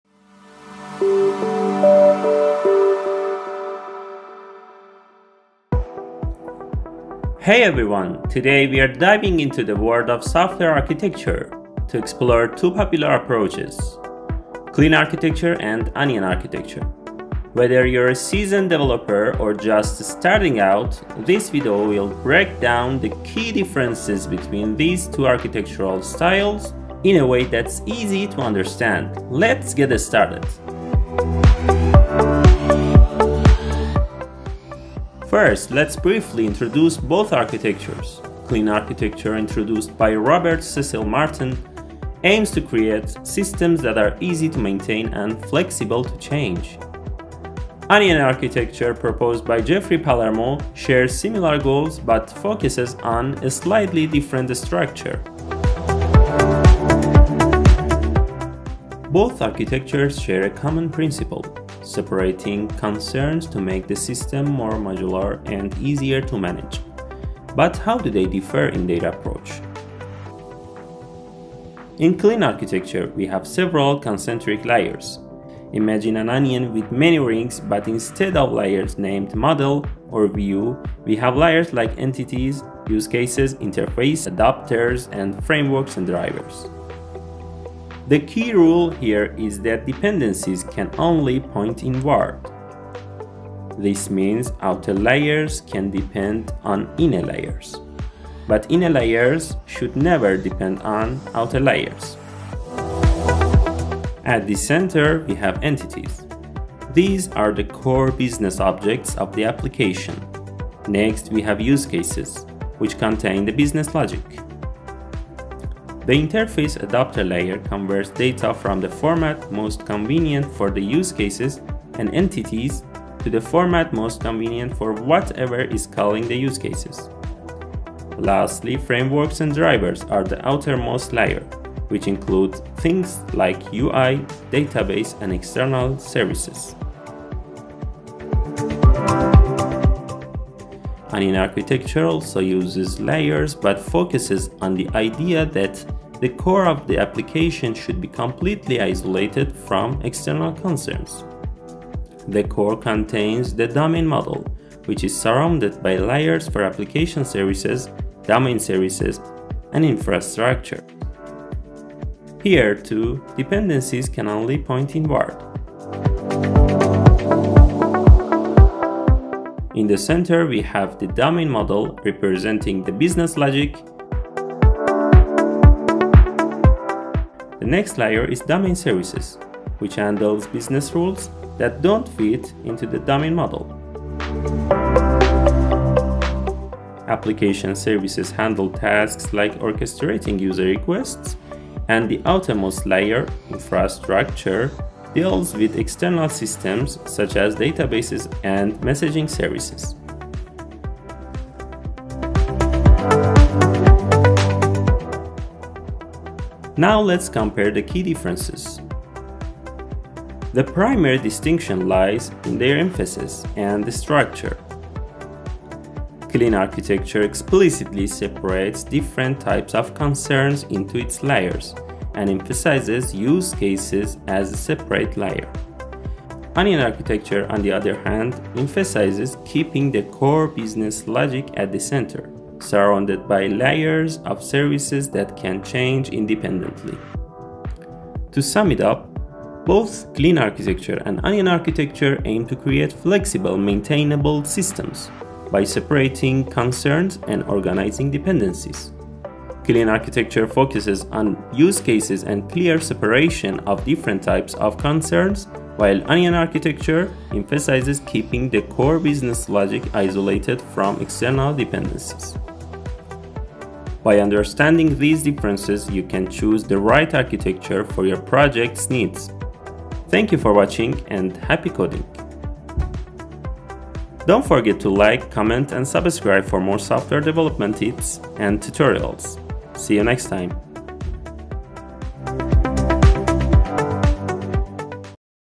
Clean Architecture vs Onion Architecture
In this video, we dive into two popular software architectures: Clean Architecture and Onion Architecture. We'll break down the core concepts of each approach, compare their key differences, and use real-world analogies to make the comparison easy to understand. Whether you're a seasoned developer or just starting out, this video will help you grasp the essentials of these architectures and decide which one might be the best fit for your next project.